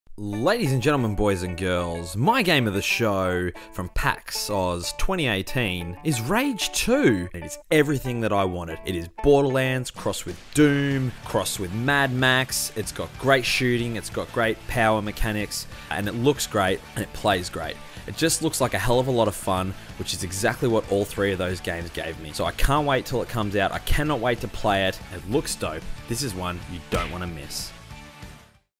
Best of PAX 2018 - Rage 2
Members of the ATEBITCollective share their thoughts on what was best in show at PAX Australia 2018, Jack Kruse's pick? None other than Rage 2.